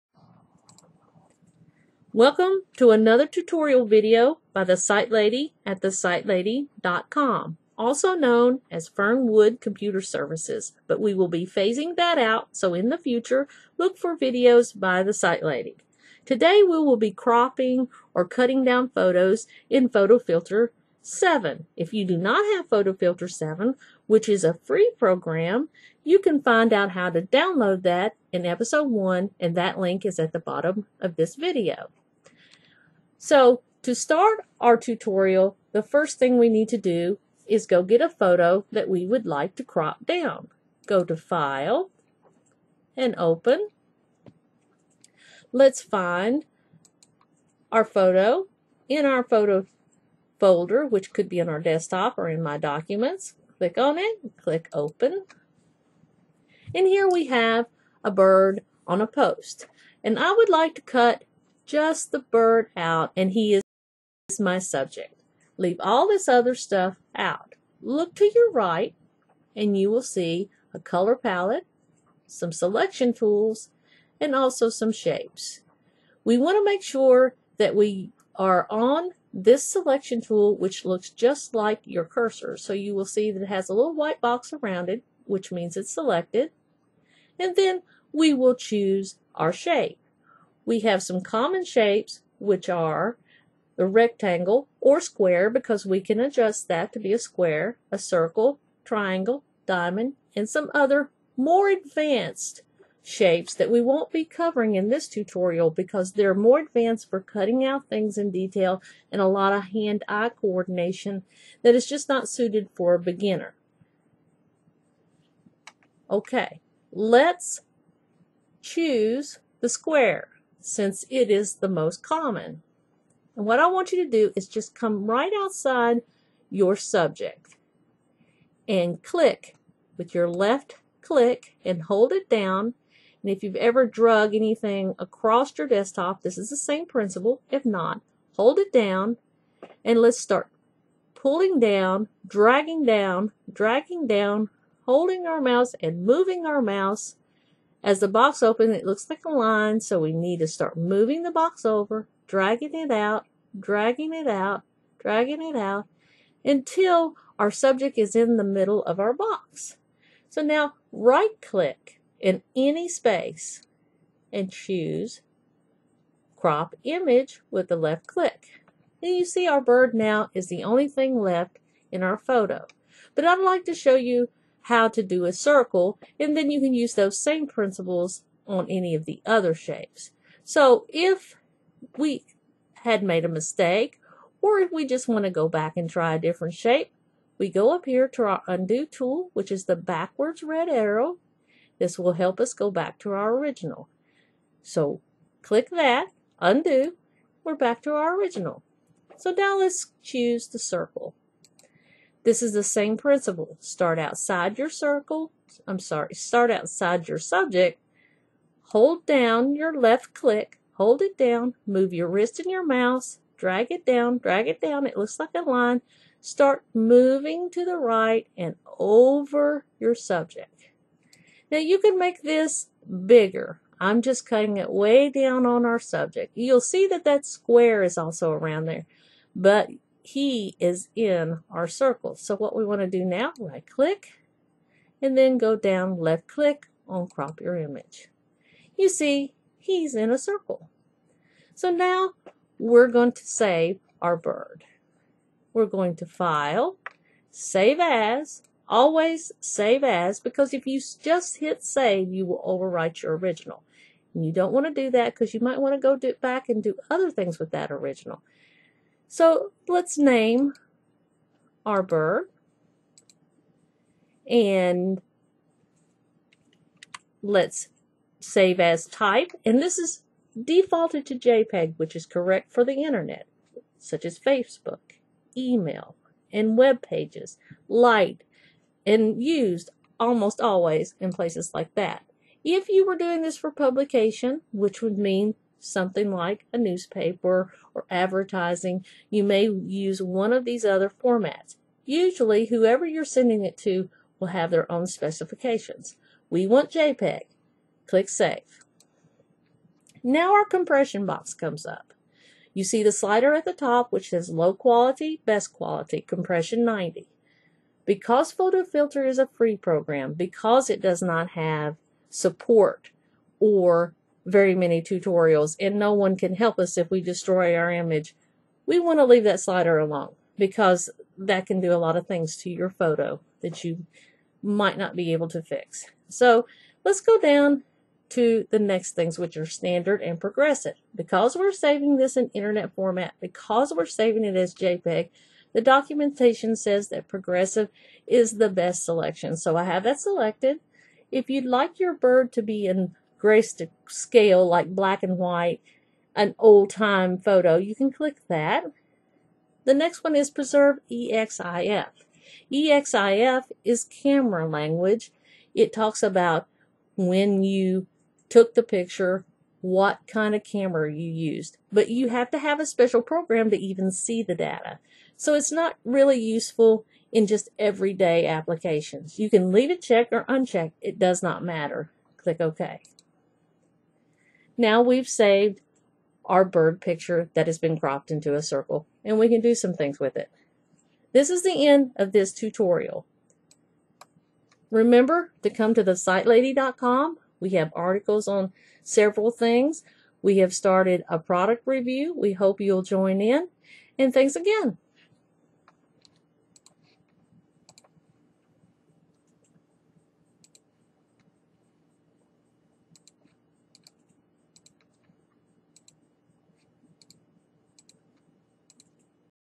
Episode 2: Cropping Photos in Free Photofiltre (English)
How to crop or cut pictures in the Free Program Photofiltre. This is the second in the series. Great clean program for beginners and even advanced photo enthusiasts. This is a beginners' tutorial. Also see